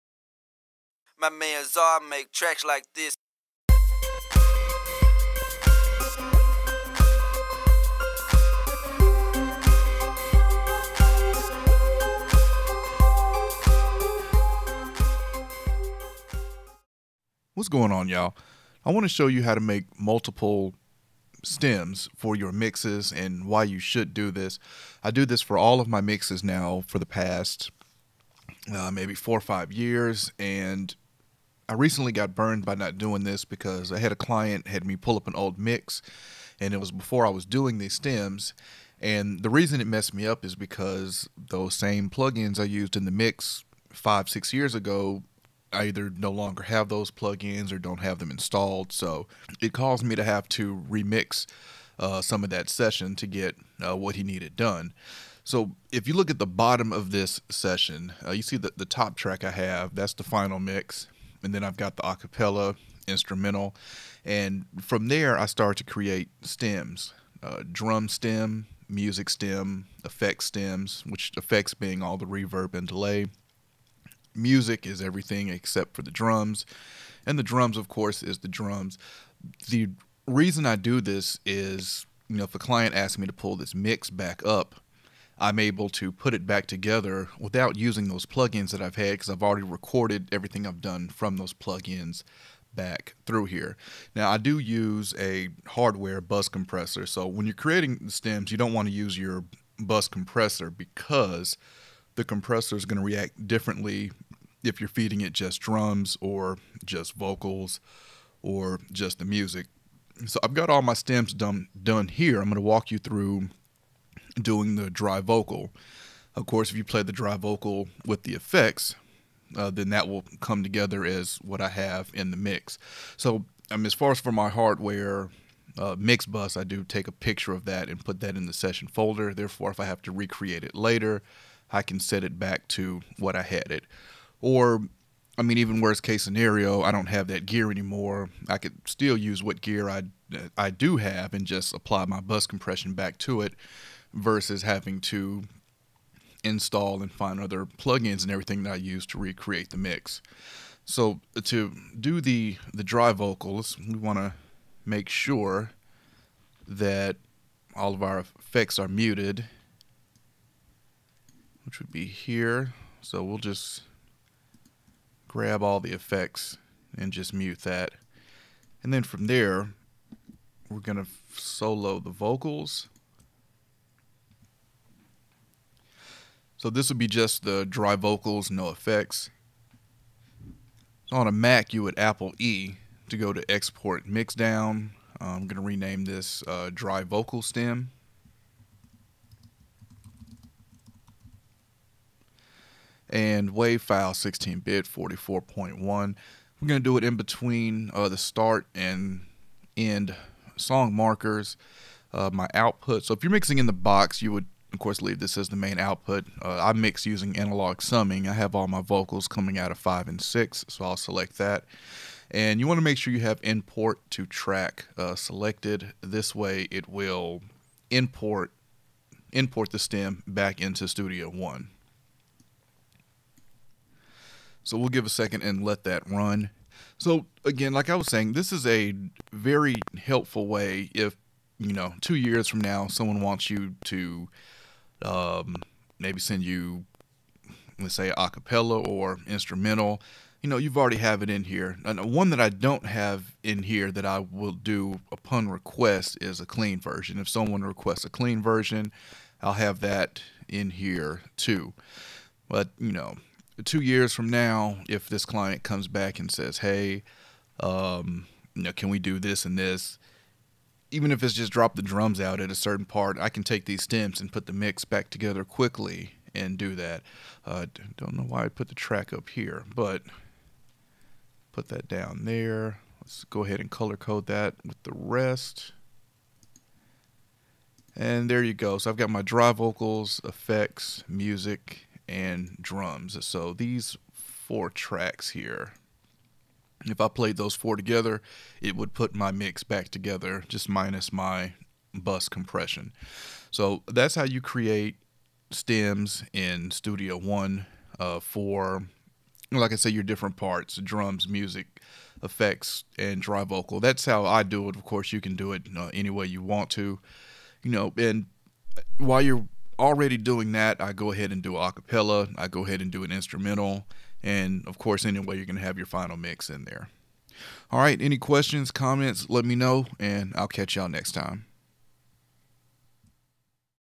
Creating Stems for your mixes in Presonus Studio One 3
This video shows how and why you should create stems once you’ve finished a mix.

save 7% on your first year of DistroKid by using my